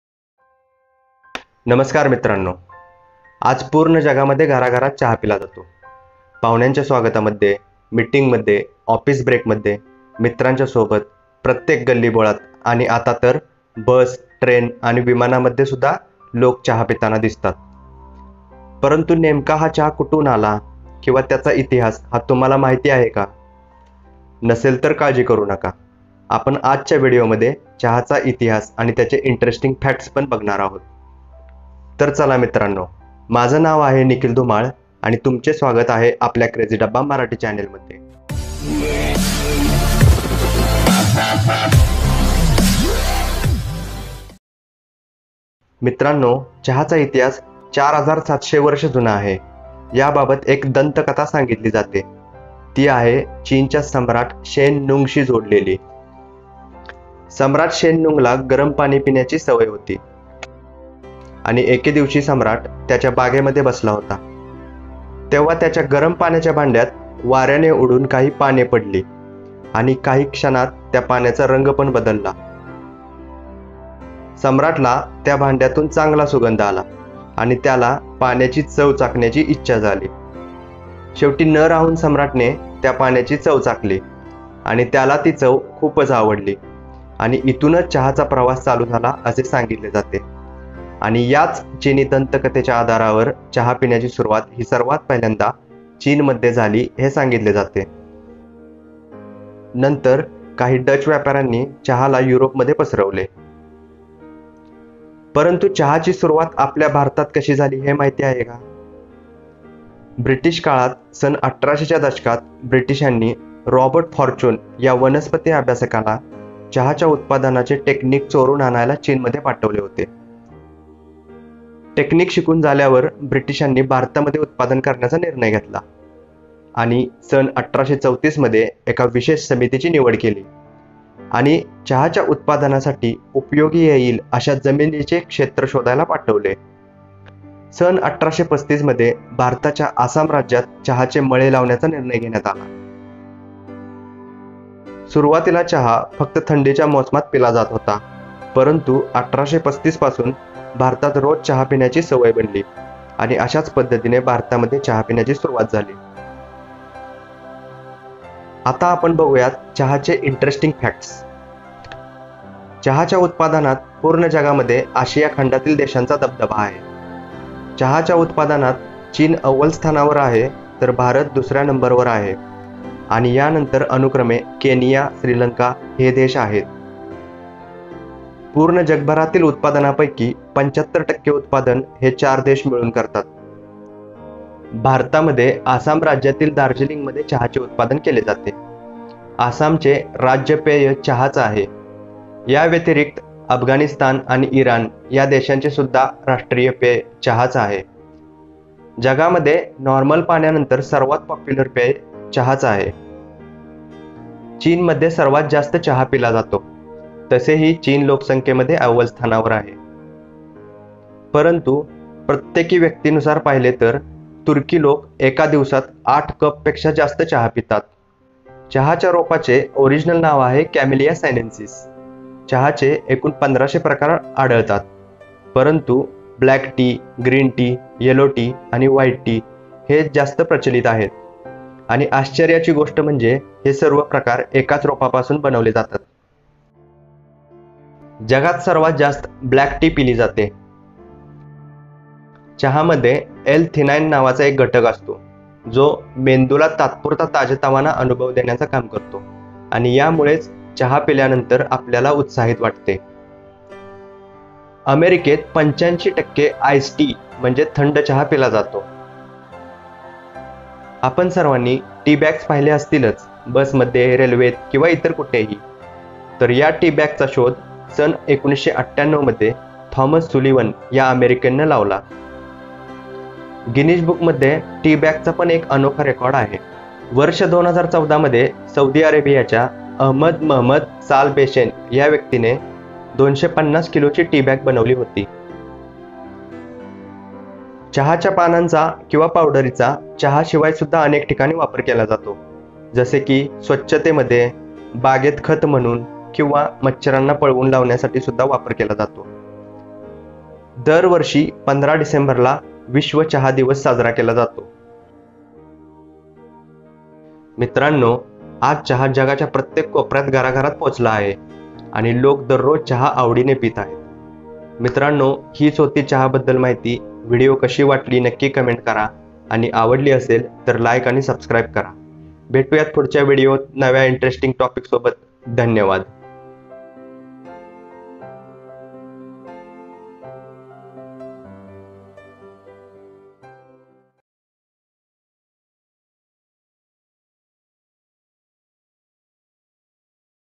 चहाचा इतिहास आणि फॅक्ट्स | Tea History And Facts In Marathi
या व्हिडिओ मध्ये आम्ही चहाचा इतिहास बद्दल माहिती दिली आहे, जसे की चीनच्या सम्राट शेन नुंग संबधित दंतकथा ते ब्रिटिशकालीन भारतातील चहा चा प्रवास, उत्पादन पद्धती आणि जगातील एकूण उत्पादन बद्दल माहिती आणि तसेच काही तथ्य किंवा इंटरेस्टिंग फॅक्ट्स सांगितले आहे उदा. चहाच्या रोपाचे नाव, चहाचे वेगवेगळे प्रकार जसे की ग्रीन टी, ब्लॅक टी आणि इतर, टी बॅग्स चा शोध आणि गिनिज बुक मधील रेकॉर्ड अशा पद्धतीची माहिती सांगितली आहे

In this Video, we bring you history about Tea and interesting facts like china king Shen Nung story about tea, british india tea journey, Tea production countries in the world, Tea plant information and types, and many more facts.